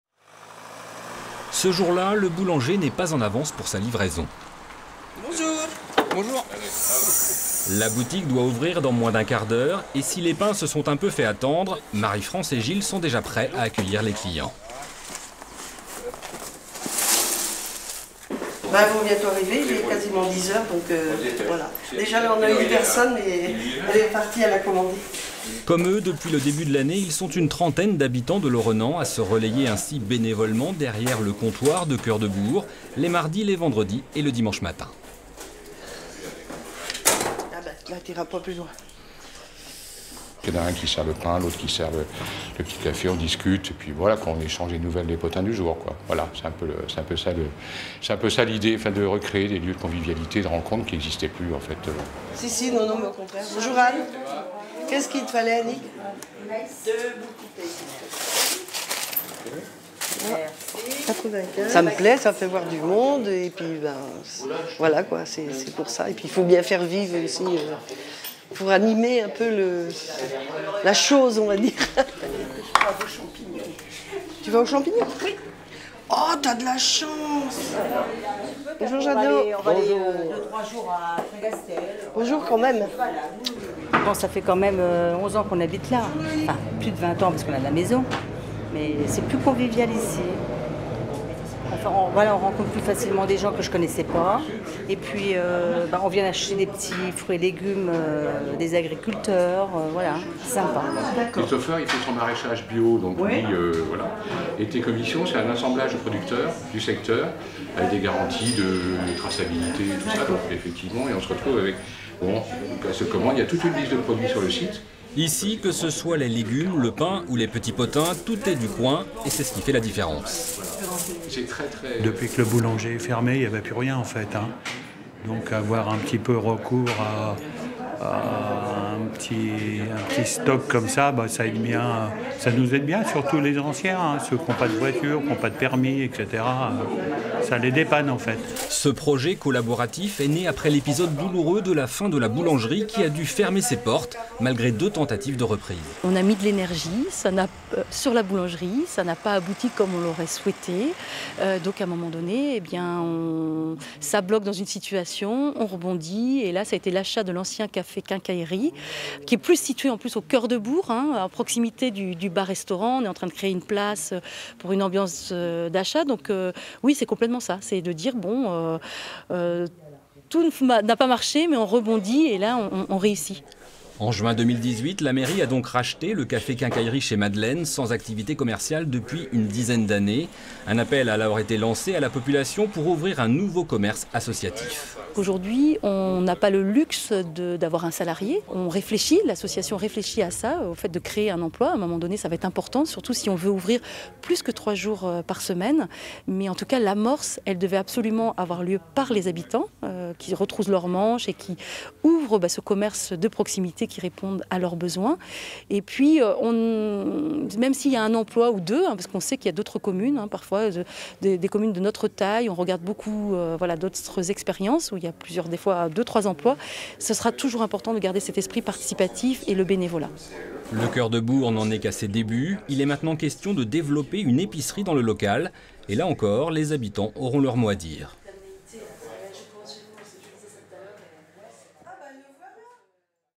"Cœur de Bourg" : un dépôt de pains, épicerie
Laurenan, petite commune des Côtes d'Armor de 730 âmes.
Là-bas, la municipalité et les habitants ont uni leurs forces pour redonner vie à un commerce de proximité. Depuis le début de l’année, un ancien café racheté par la commune fait office de dépôt de pain, d’épicerie, et a vocation à se développer. De quoi recréer du lien social dans le village.
Reportage : Fabrice Leroy, Jean-Michel Piron, Hélène Notat.
Intervenants : Marie-France Rozé (Bénévole depuis l'ouverture), Gilles Cochet (Nouveau bénévole), Hélène et Dominique  Heu (Habitants du bourg) et Valérie Poilâne-Tabart (Maire de Laurenan).